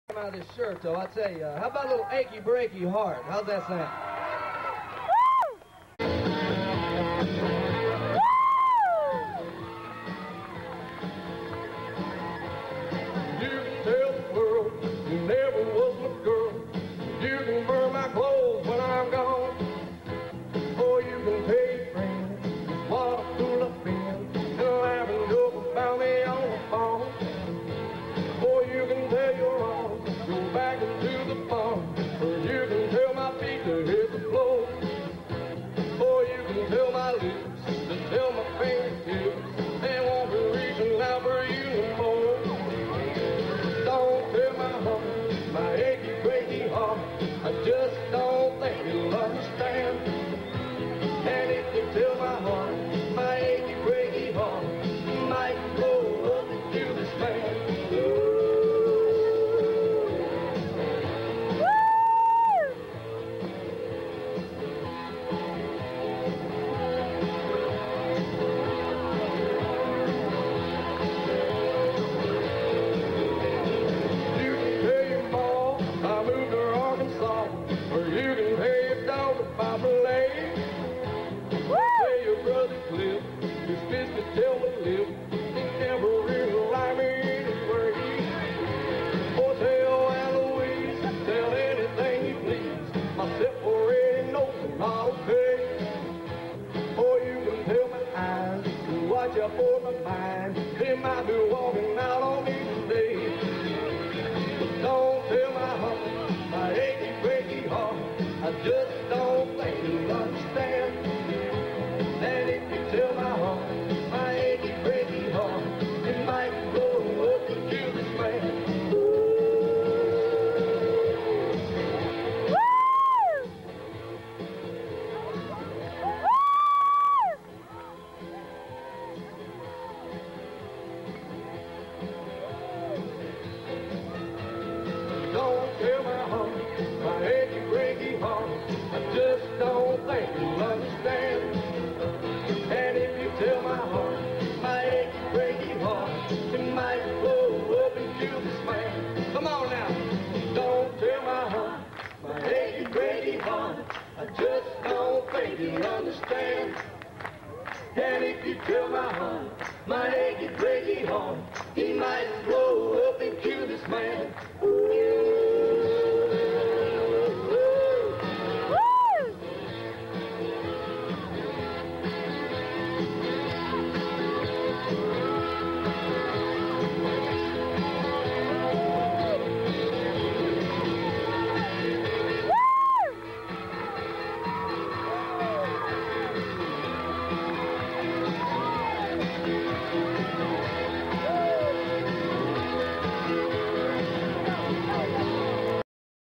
Billy Ray Cyrus Impersonator
My good friend David Eagle, the BEST Billy Ray Cyrus Impersonator